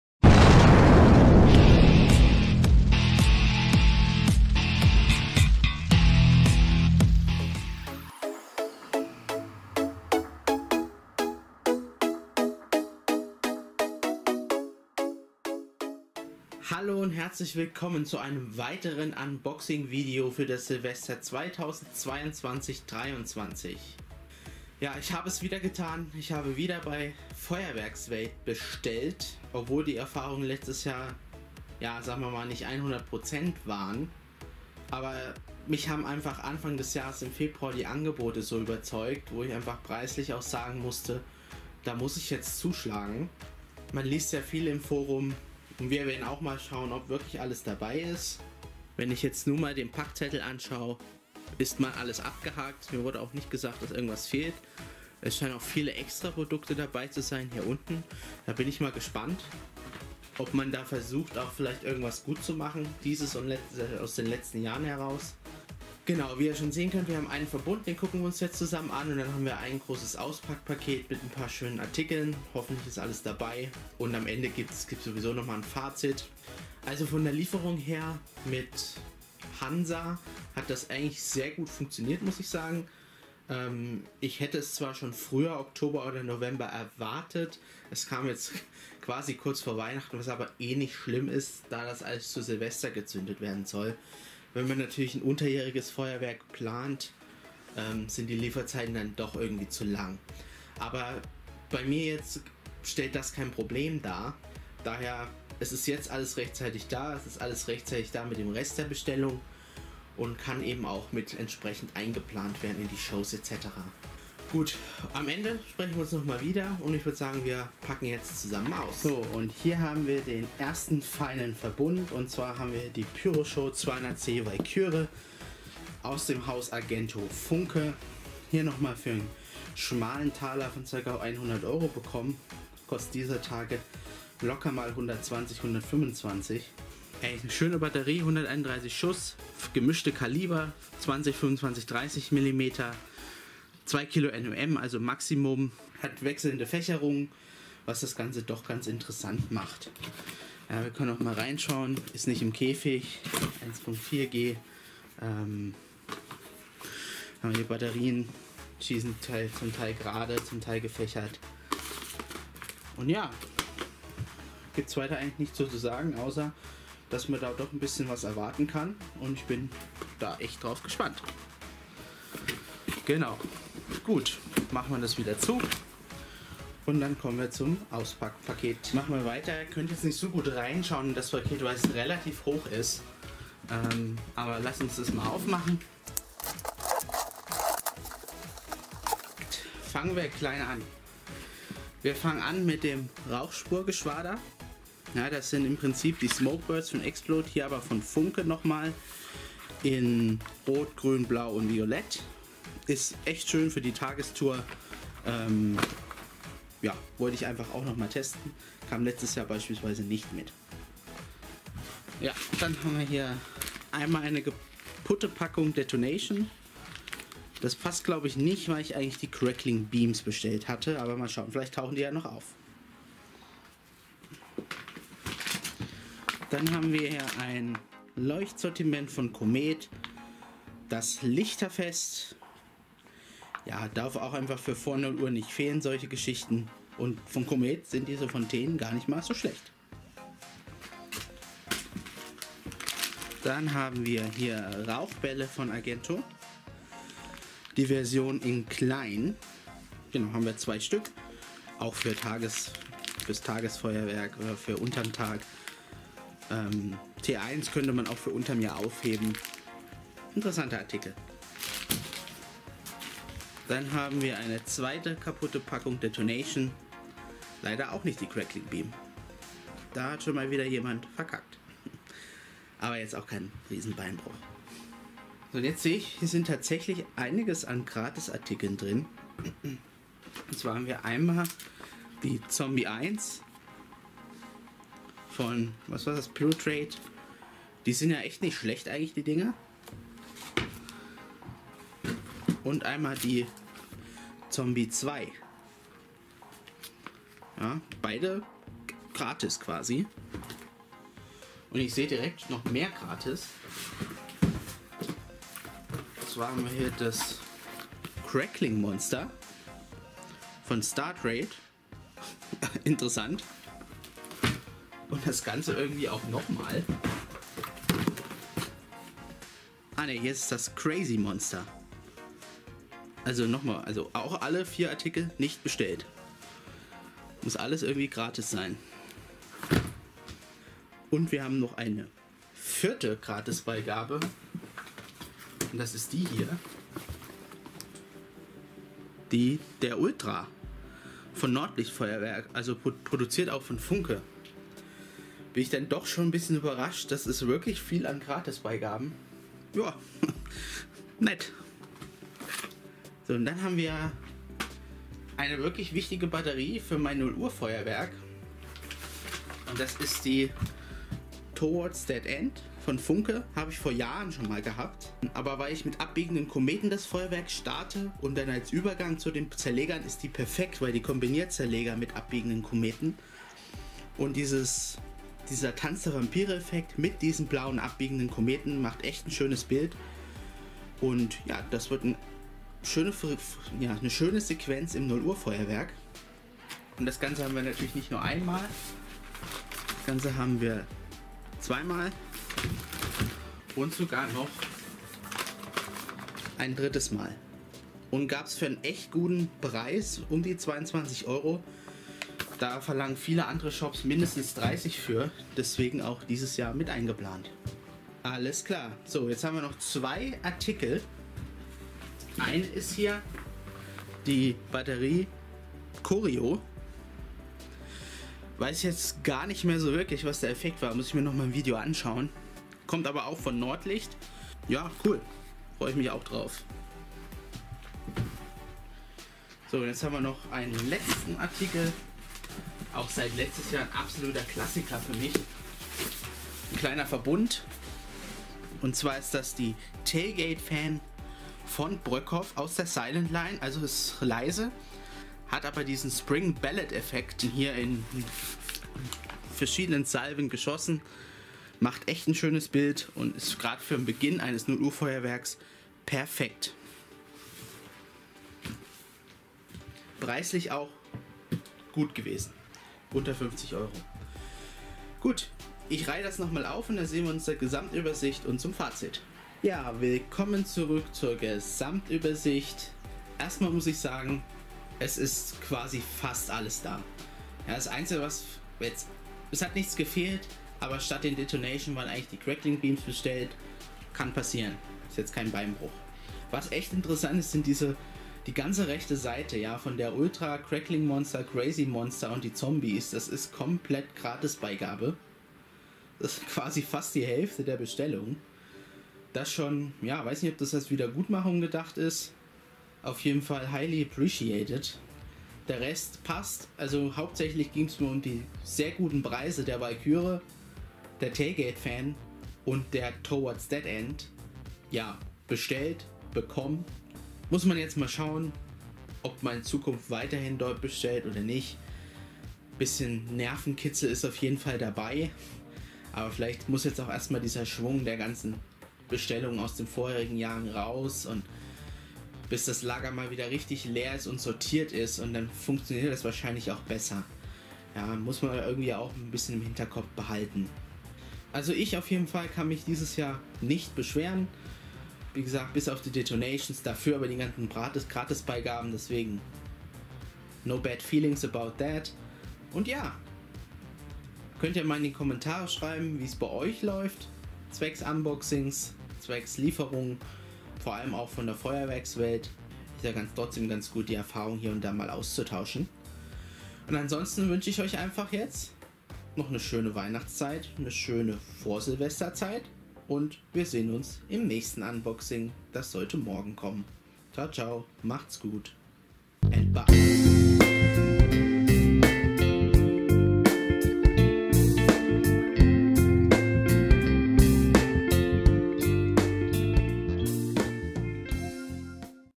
[Feuerwerkswelt] | Silvester Unboxing 2.0 | Wie lief es 2022?
Unboxing V Silvester 2022/23

Jeder verdient eine 2. Chance - nachdem die Lieferung letztes Jahr zu spät für Planung des 0-Uhr Feuerwerkes kam habe ich trotzdem wieder bestellt - hauptsächlich wegen den guten Preisen Anfang des Jahres.

Bestellt mit Befähigung nach Paragraph 27.

Fazit:
Angebot & Preise : Top!
Handhabung/Support/Lieferung: Immer noch nicht wo es sein muss!
Lieferant Hansa Logistik hat aber wunderbar funktioniert!